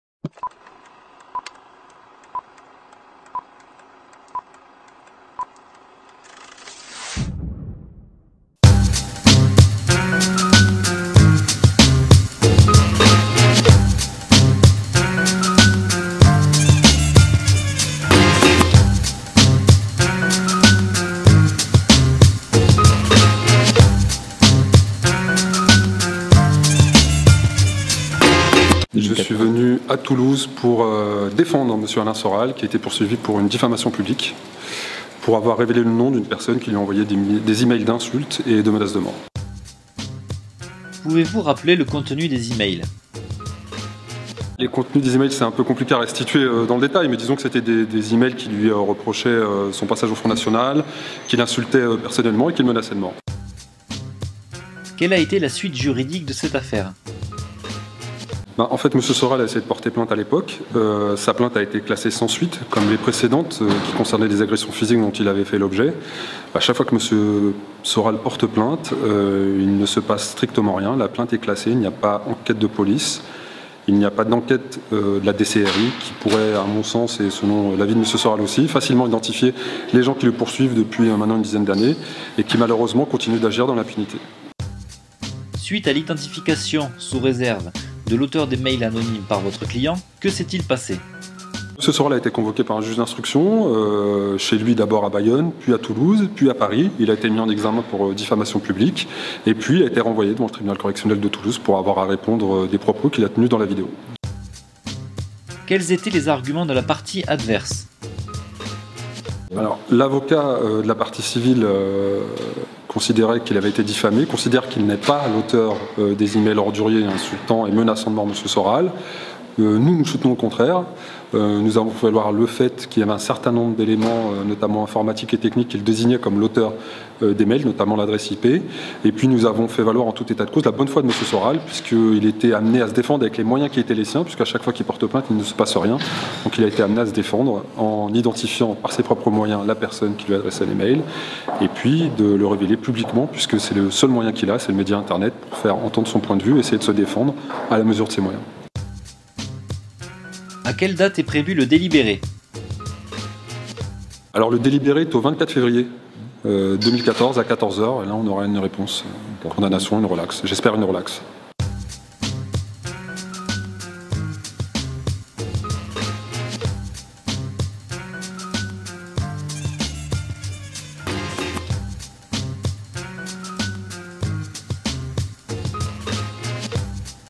Procès Alain Soral / Employé de la mairie de Toulouse : réaction de Me Drici (3 février 2014)
Lundi 3 février 2014, au Palais de justice de Toulouse, se tenait l'audience opposant Alain Soral à un employé de la mairie de Toulouse.

Suite à des menaces de mort reçues par un anonyme sur internet, Alain Soral est parvenu à identifier l'auteur présumé des emails litigieux.

Après la vidéo de l'entretien d'Alain Soral de novembre 2011 (partie 2),  l'employé de la mairie de Toulouse a déposé plainte pour diffamation, injure publique et atteinte à la vie privée.

Me Lahcène Drici réagit à la fin de l'audience de plaidoiries.